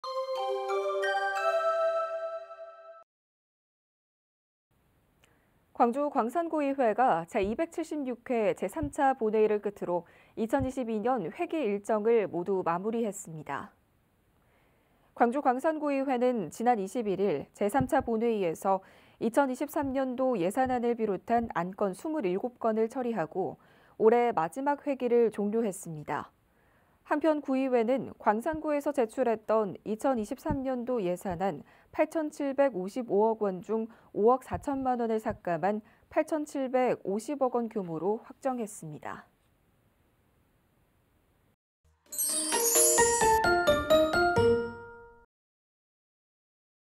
[광주뉴스] 광주 광산구의회, 제276회 정례회 폐회...예산 8,750억 원 확정
■CMB뉴스

광주 광산구의회가 제276회 제3차 본회의를 끝으로
2022년 회기 일정을 모두 마무리했습니다.

광주 광산구의회는 지난 21일
제3차 본회의에서 2023년도 예산안을 비롯한 안건 27건을 처리하고,
올해 마지막 회기를 종료했습니다.

한편, 구의회는 광산구에서 제출했던 2023년도 예산안
8천 7백 55억 원 중 5억 4천만 원을 삭감한
8천 7백 50억 원 규모로 확정했습니다.